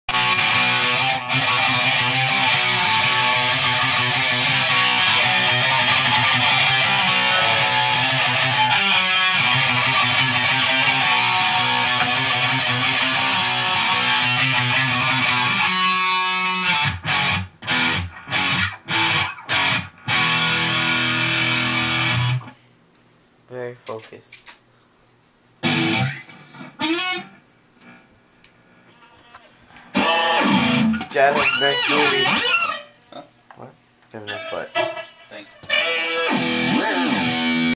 My butt
Playing around on my friends sexy Ephiphone Les Paul. Super sweet, nice sound.